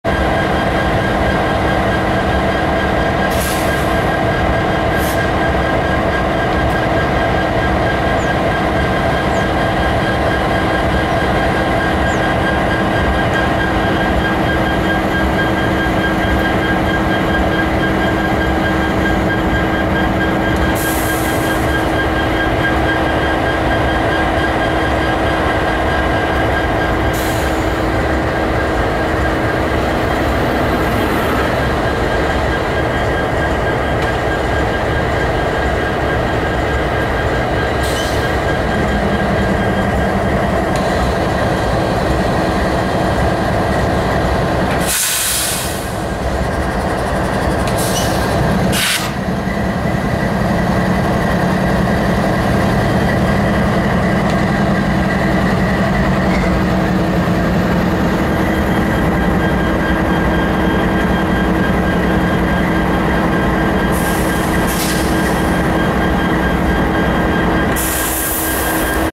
10/28/11 Testing out my new HD Camera
NS 9067 D9-40CW, NS 9862 D9-40CW, BNSF 9488 SD70MAC, and BNSF 4673 D9-44CW sit while I get a perfect test video for my new camera